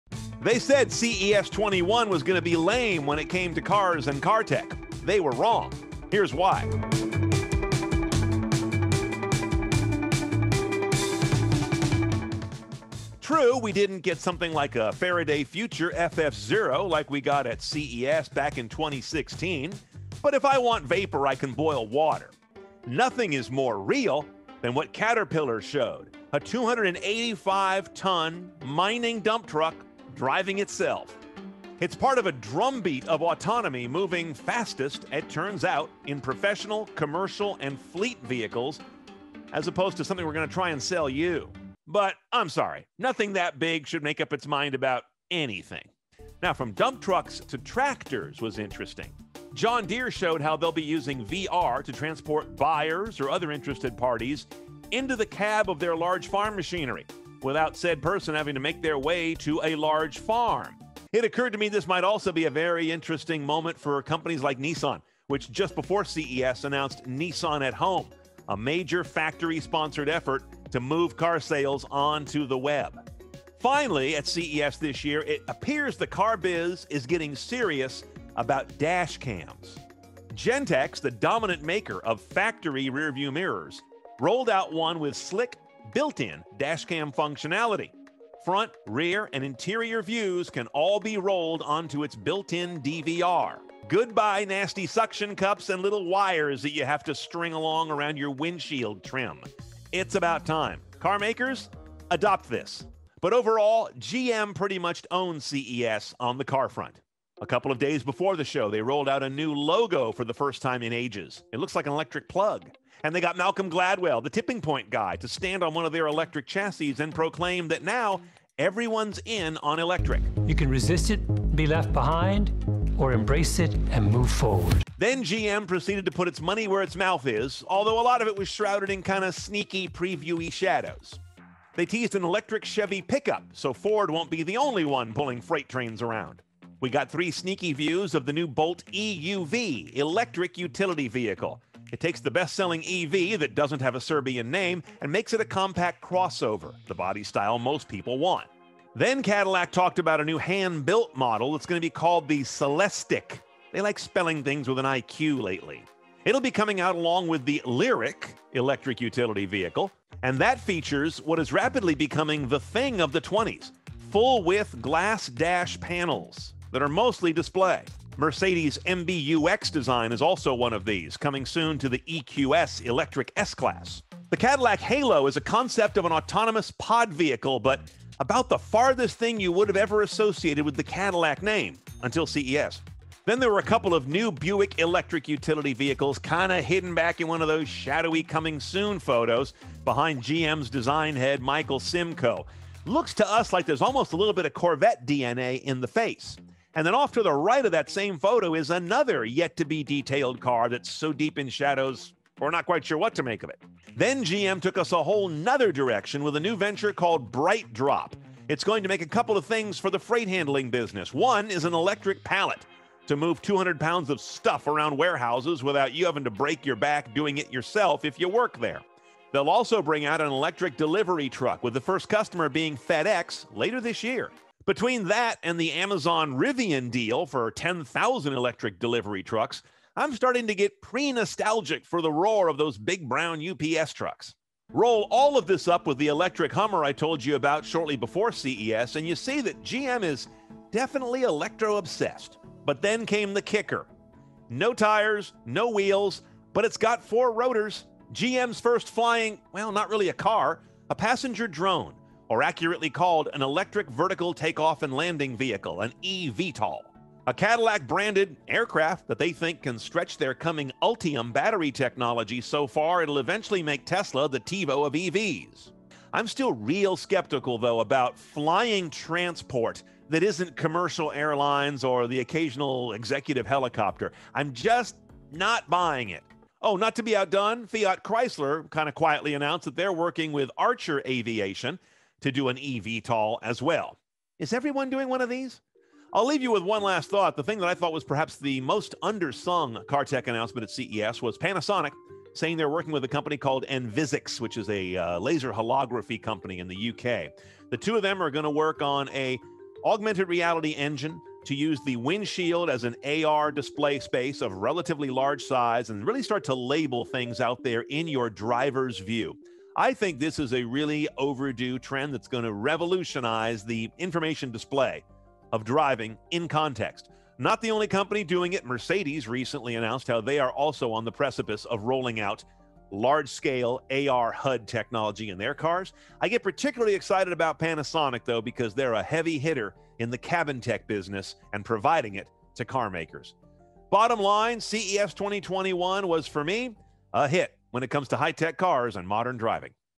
General Motors made the most of CES 2021, betting the farm on a new idea
Everyone’s in, goes the new refrain, as GM slides all its chips across the table toward electrification with a slew of sneak peeks at their fleet of coming electric cars, and one that isn’t a car at all.